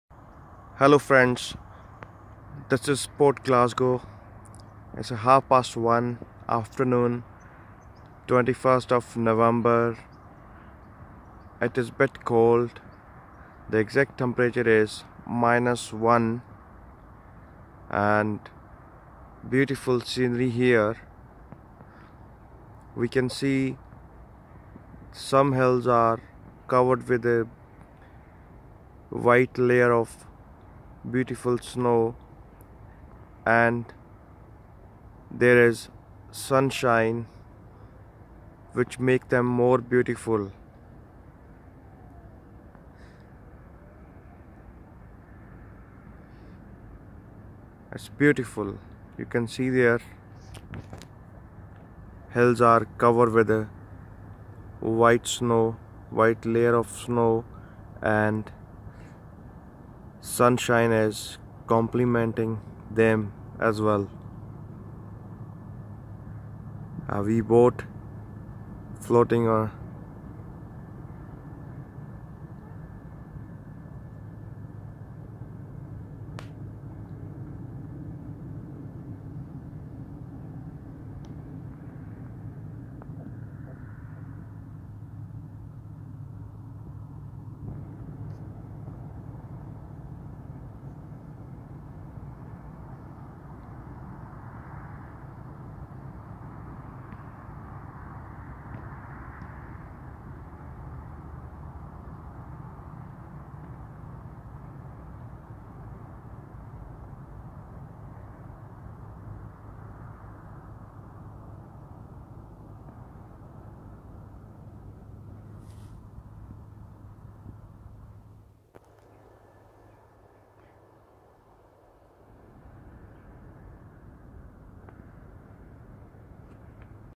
Glasgow||Scotland||scenery|| Natural beauty
Beautiful scenery recorded in chilly afternoon --2 degree temp at Port Glasgow in Scotland (UK). Please prescribes for more exciting videos.